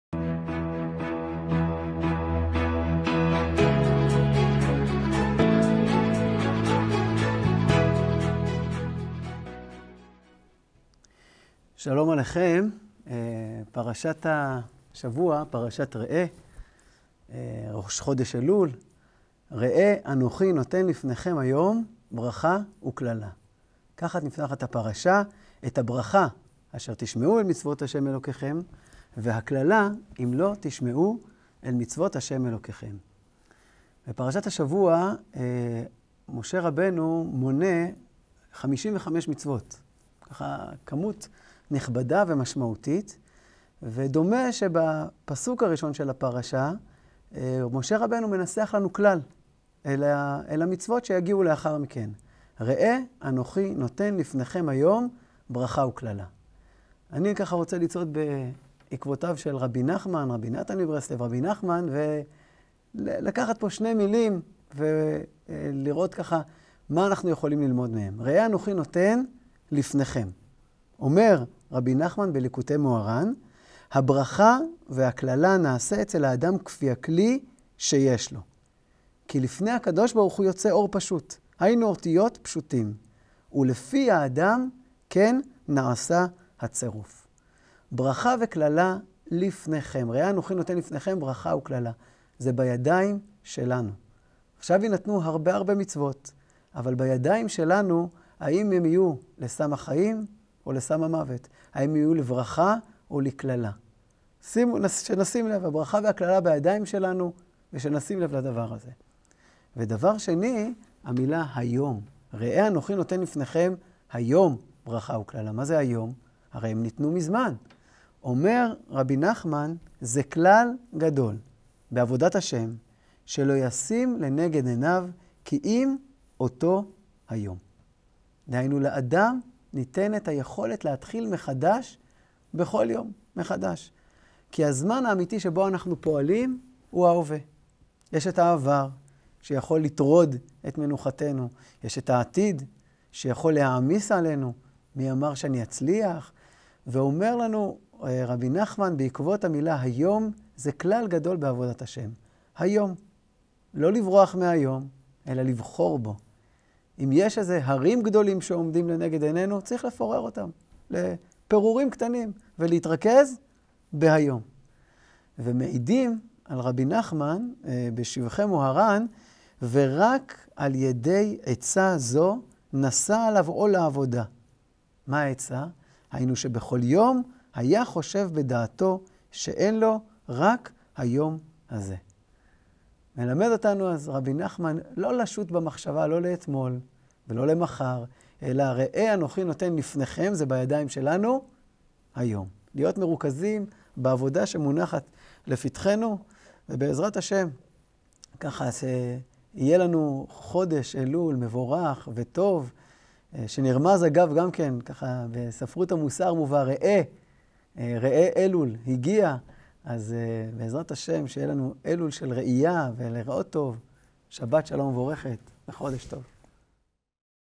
'ההקדמה לקיום המצוות' - דבר תורה לפרשת ראה | הרב עוז מסוורי
בעקבות רבי נחמן מברסלב נבקש לראות בפסוק הראשון כלל חשוב בעבודת השם.

חפשו גם 'ישיבת הגולן' בספוטיפיי להאזנה לפודקאסטים של הישיבה.